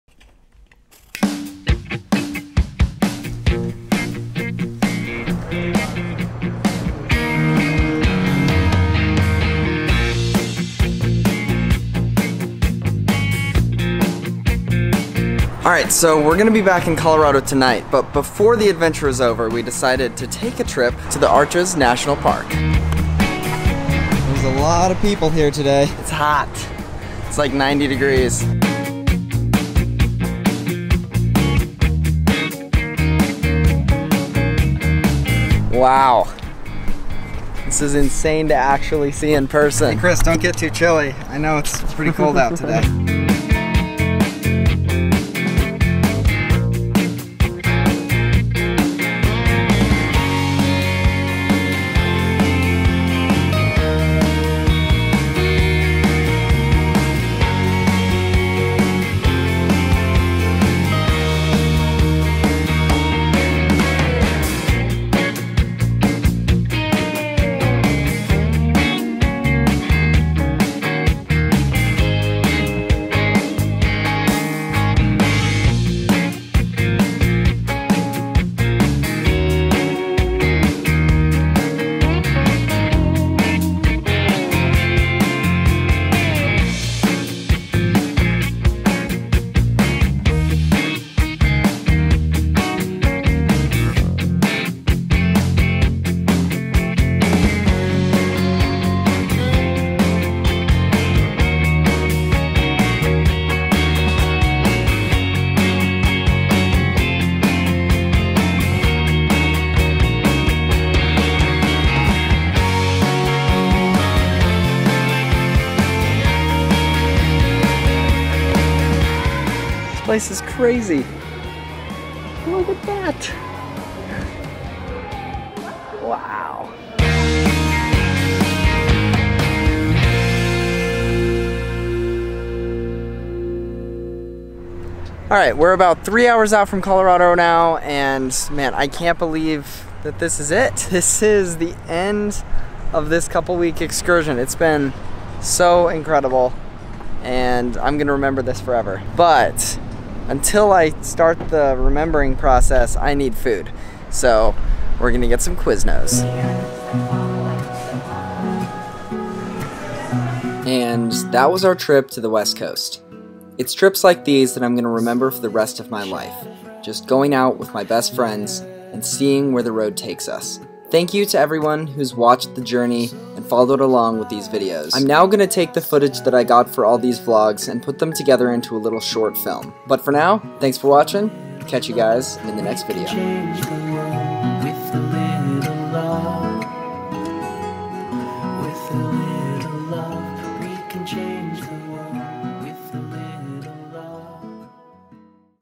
WELCOME TO RADIATOR SPRINGS!
Today we explored the Arches National Park and arrived home in Colorado Springs.

Filmed with a Sony a6300 / Canon 60D / iPhone 8.
Edited with Final Cut Pro X / Adobe After Effects.